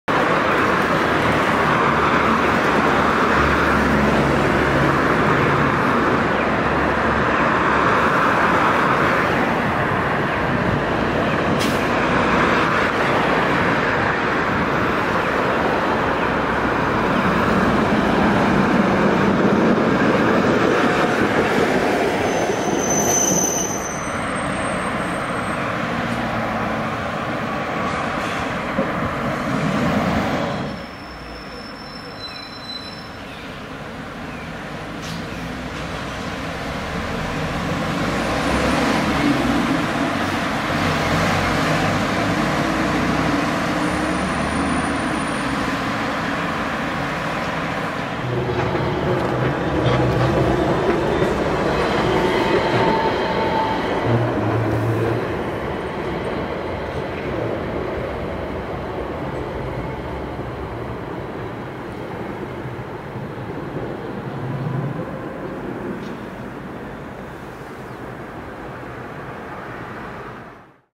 熊本市交通局 0800形 超低床路面電車
熊本市交通局（熊本市電）のブレーメン形超低床電車、0800形です。

2014年10月3日に、この0800形の特別車 "COCORO"（水戸岡鋭治氏のデザイン）が運行開始されます。
この映像に写っている既存の0800形も、それに合わせて塗装が変更される予定です。